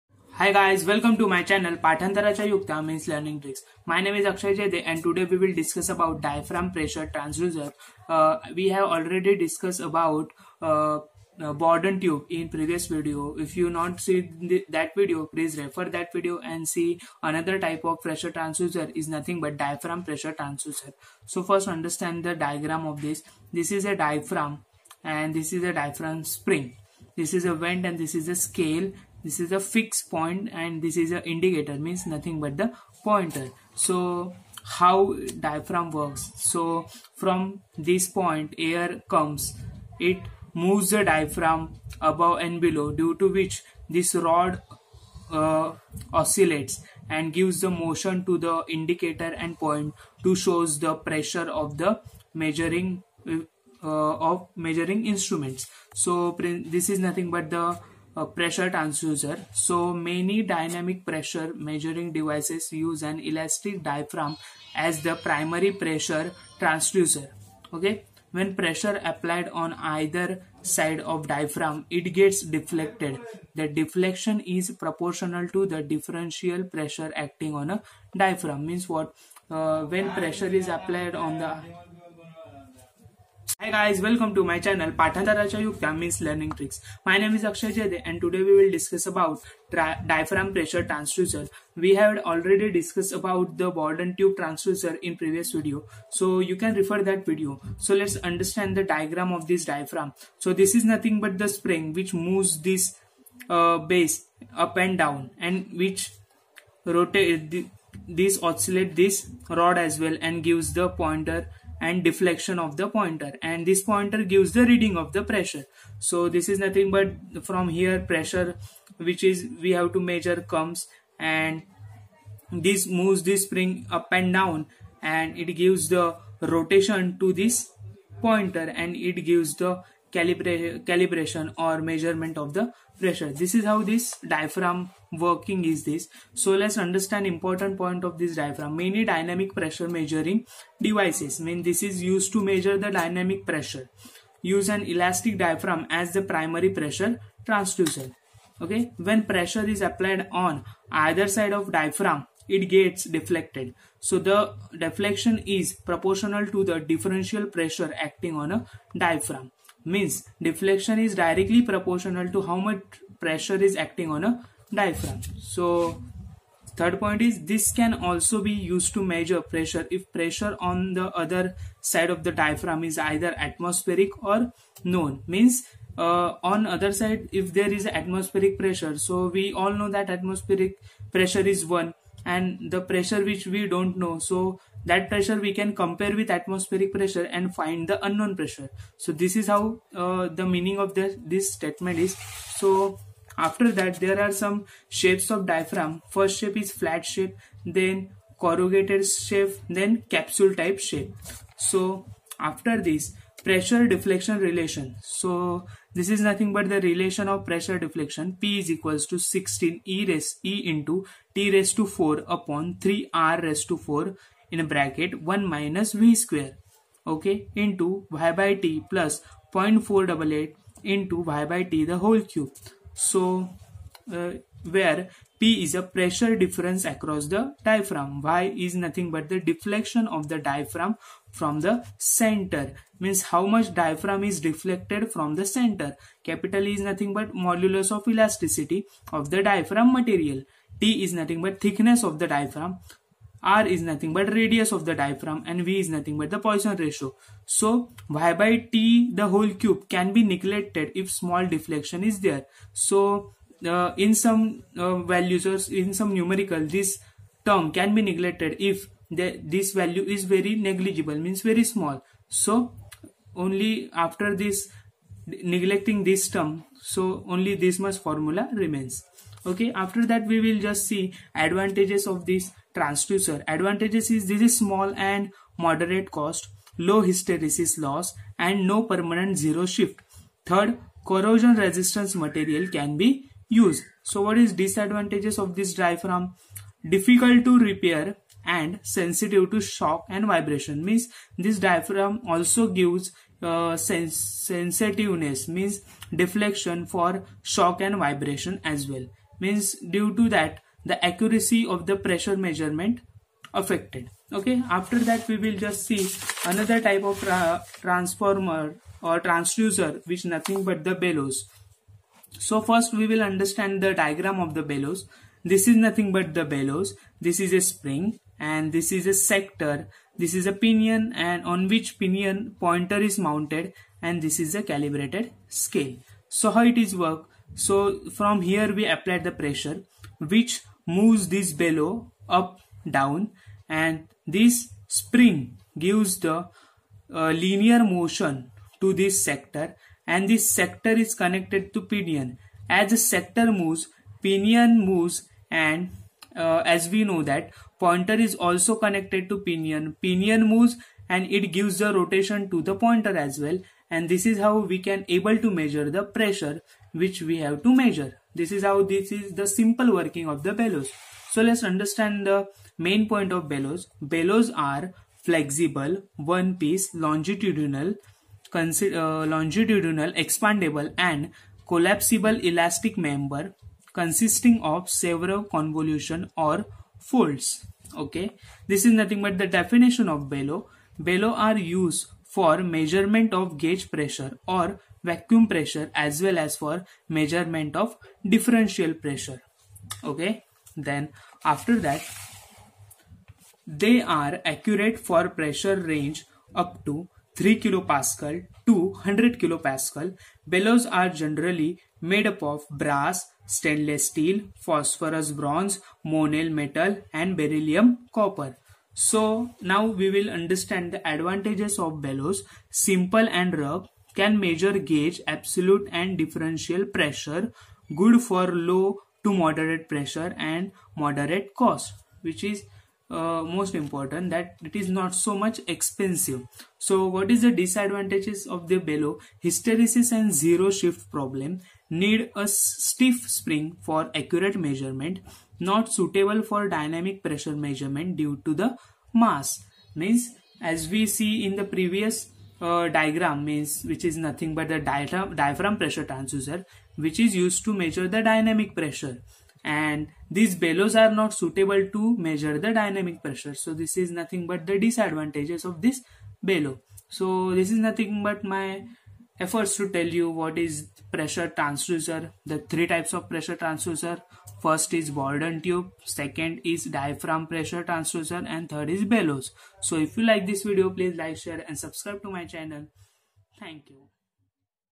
Bellows and diaphragm pressure transducer. || Jedhe Online Class
This video is all about working advantage,disadvantage of diaphragm pressure transducer and bellows.